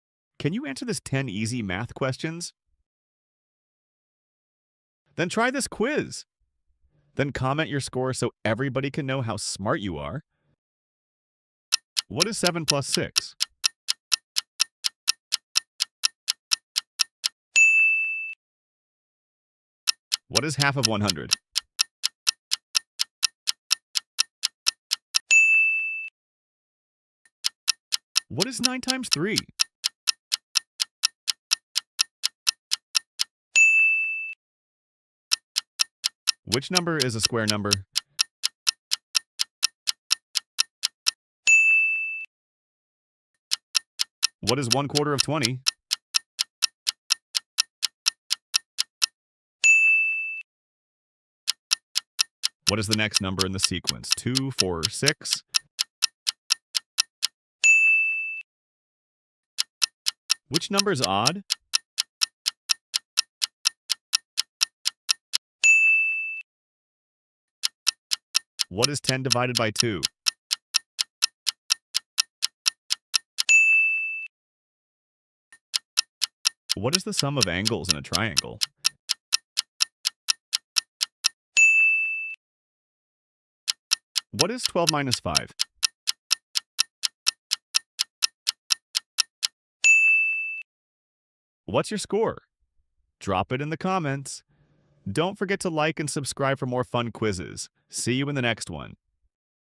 10 EASY MATH QUESTIONS
🧠 Think you're good at math? Try answering these 10 EASY math questions and see how fast you can solve them! Perfect for students, math lovers, or anyone who wants to sharpen their brain with quick challenges.

✅ Great for all ages
✅ Boost your math skills
✅ Fun, fast, and educational!